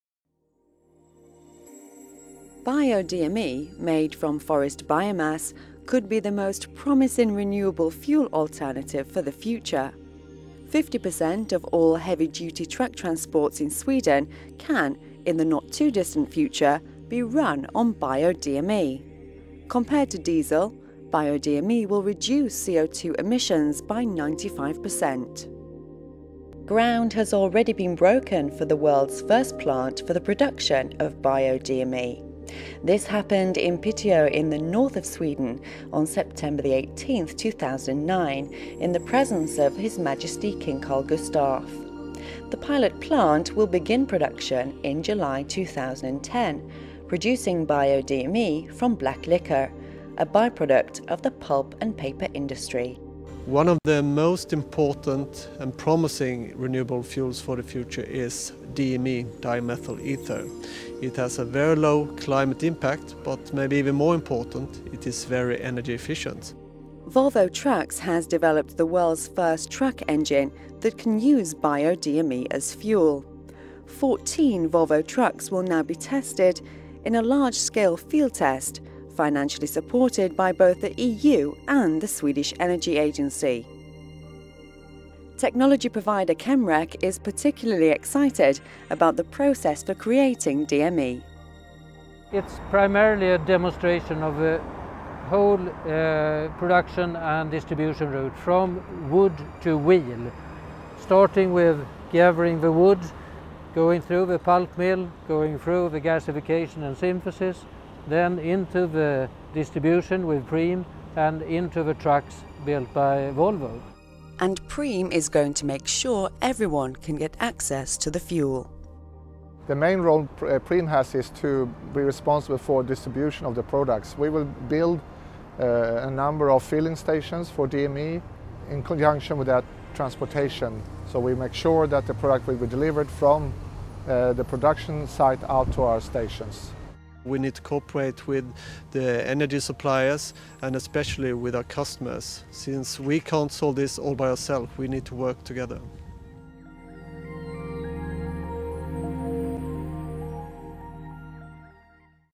Volvo Trucks - Bio-DME trucks from Volvo in unique field test
In 2010, Volvo Trucks will be the first truck manufacturer to start conducting comprehensive field tests involving Bio-DME. This is a biofuel that generates very low carbon dioxide emissions. In the long term it has the potential to replace 50 percent of todays diesel use for transport operations in Europe.